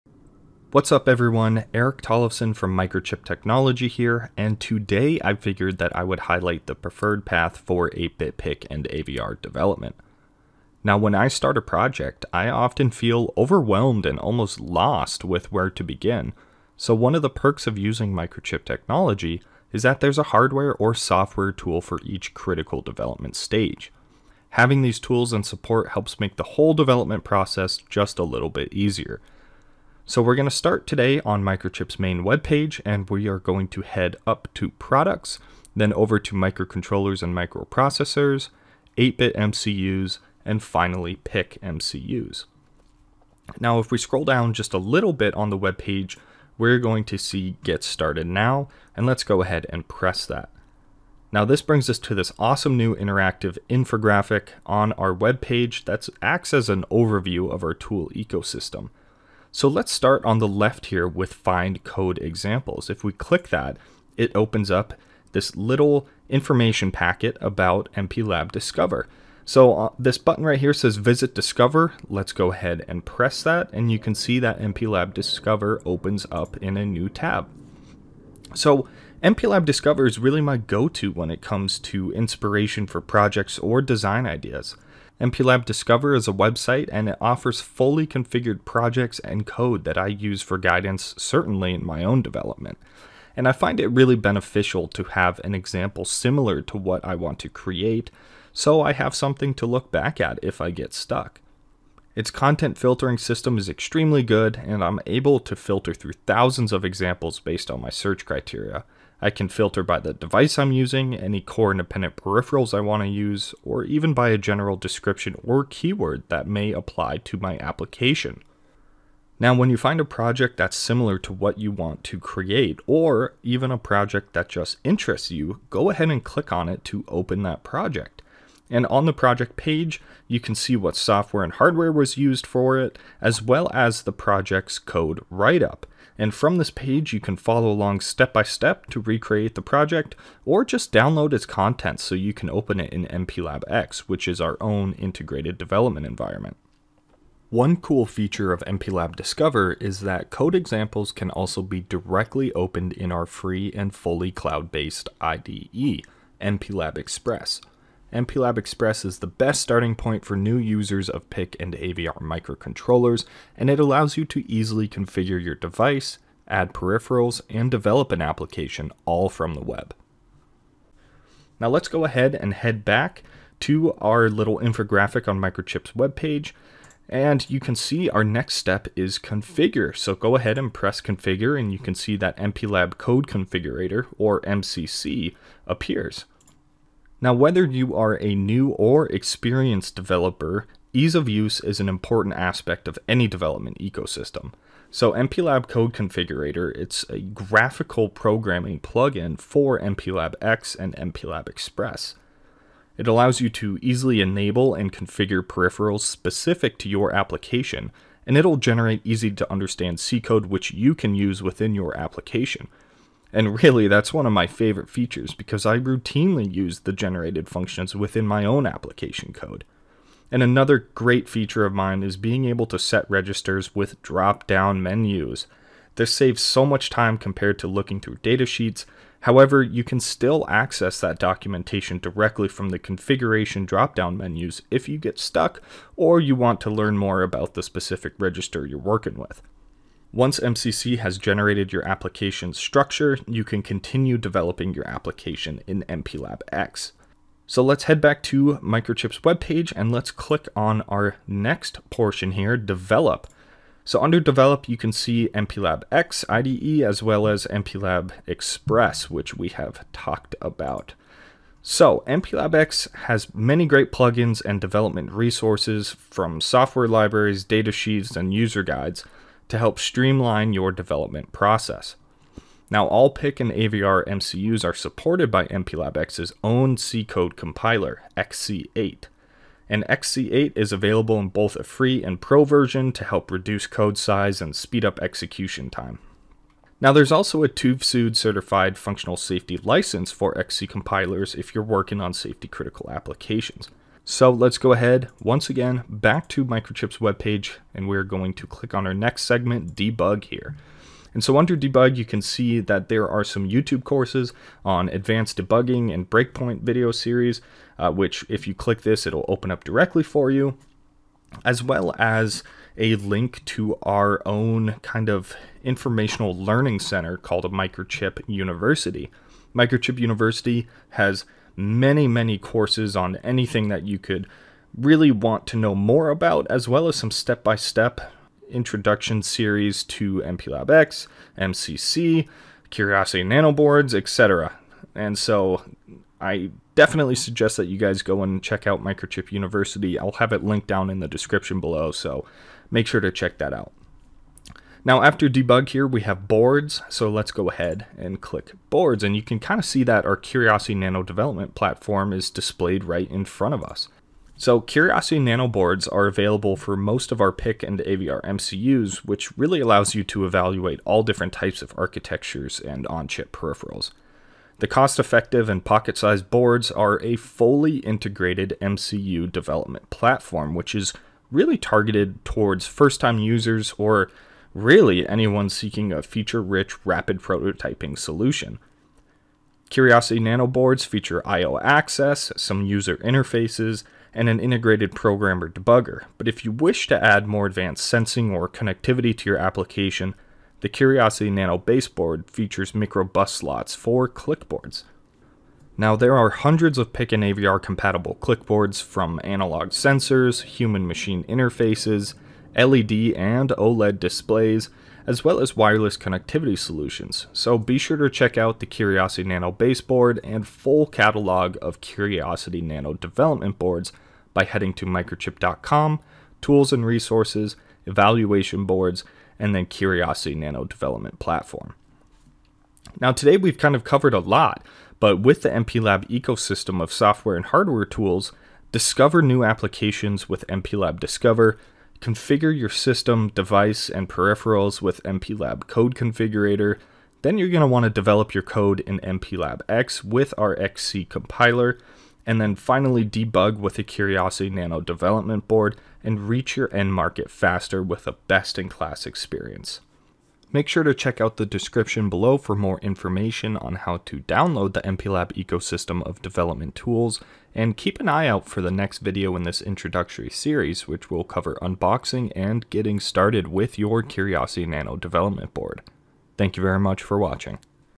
PIC® and AVR® Hardware and Software Development Process
This video outlines the preferred path for 8-bit PIC and AVR development using the MPLAB ecosystem of software and hardware tools.
Please see the listed resources below to begin your development process today!

Tools and Resources:
Getting Started Tutorials: